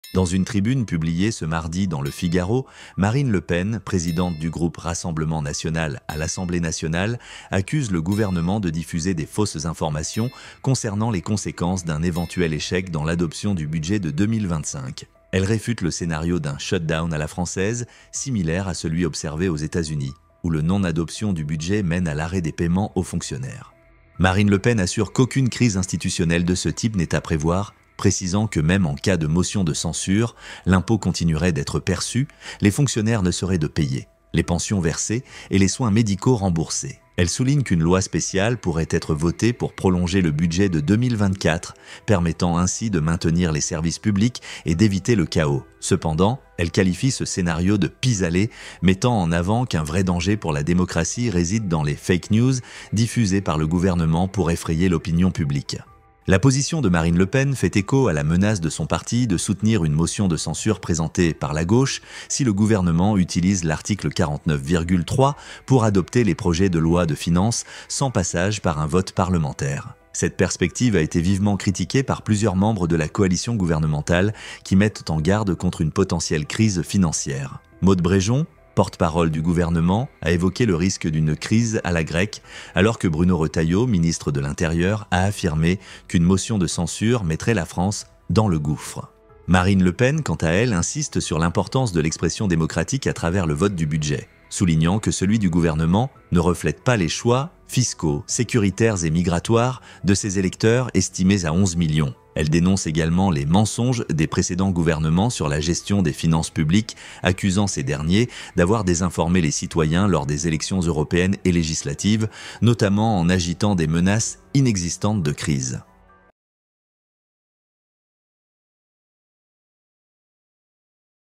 Marine Le Pen Explose De Colère : Le Gouvernement Ment sur le Paiement des Fonctionnaires!
Dans une tribune publiée Le Figaro, Marine Le Pen accuse le gouvernement de propager des "fausses informations" sur les conséquences d’un échec de l’adoption du budget 2025. Elle rejette l'idée d'un "shutdown" à la française, affirmant que les fonctionnaires seront payés et que les services publics continueront de fonctionner, même en cas de motion de censure.

🗣️ "Les fake news du gouvernement visent à effrayer l'opinion publique", déclare-t-elle, insistant sur l’importance de maintenir la démocratie en ces temps de crise.

💬 Marine Le Pen menace de soutenir une motion de censure de la gauche si l'article 49.3 est utilisé pour faire passer le budget sans vote parlementaire. Le débat fait rage, avec des avertissements sur le risque d'une crise financière majeure.

🔑 Qu'en pensez-vous ? Le gouvernement est-il responsable dans cette situation ?